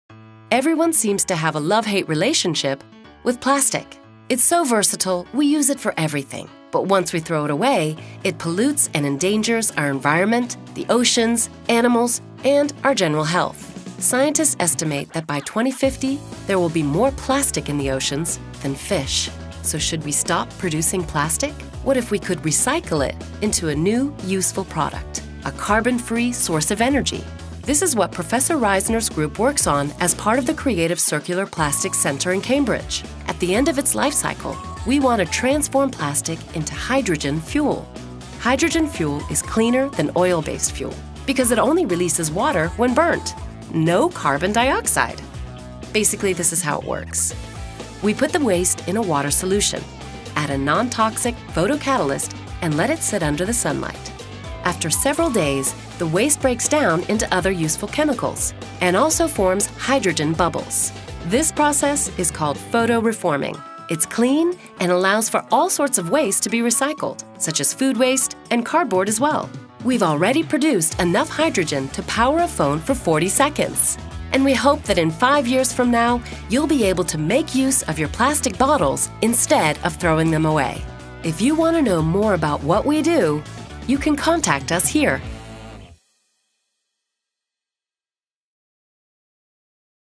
What can we do with all this plastic?
Plastic waste is a growing environmental problem, leading to ever-increasing pollution across our planet as we consume and dispose of plastic products. Currently, few good solutions exist for dealing with waste plastic, making it a difficult challenge to solve.

In the Reisner group, as part of the Creative Circular Plastics Centre at the University of Cambridge, we are developing new processes to tackle plastic waste. One of these is photoreforming, where we use sunlight to break down plastic waste, forming green fuel like hydrogen and other useful chemicals in a sustainable process. With processes like photoreforming, we hope to turn the plastic waste problem into a solution for a greener planet.

Thanks to our partners at Cartoonbase for their help with this video!